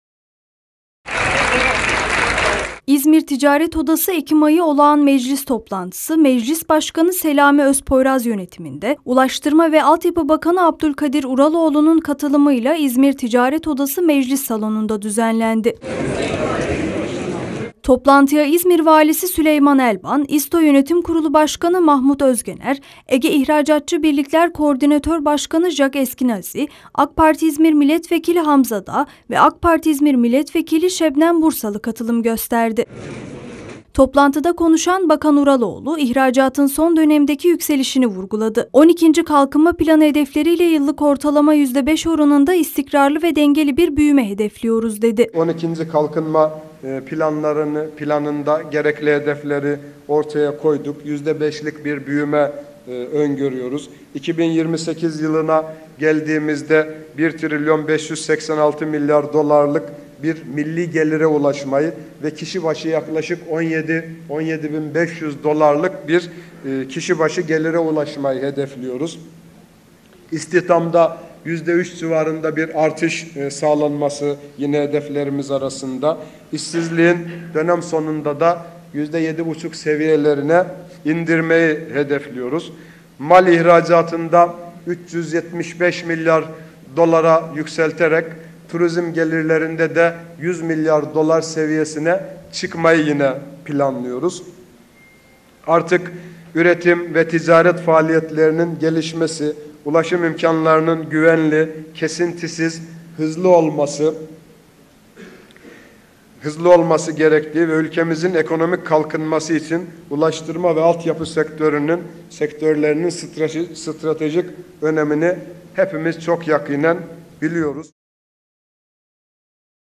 BAKAN URALOĞLU, İZTO MECLİS TOPLANTISINDA KONUŞTU
İZMİR TİCARET ODASI EKİM AYI OLAĞAN MECLİS TOPLANTISI, MECLİS BAŞKANI SELAMİ ÖZPOYRAZ YÖNETİMİNDE, ULAŞTIRMA VE ALTYAPI BAKANI ABDULKADİR URALOĞLU’NUN KATILIMIYLA İZMİR TİCARET ODASI MECLİS SALONU’NDA DÜZENLENDİ. URALOĞLU, KONUŞMASINDA ÖNEMLİ MESAJLAR VERDİ.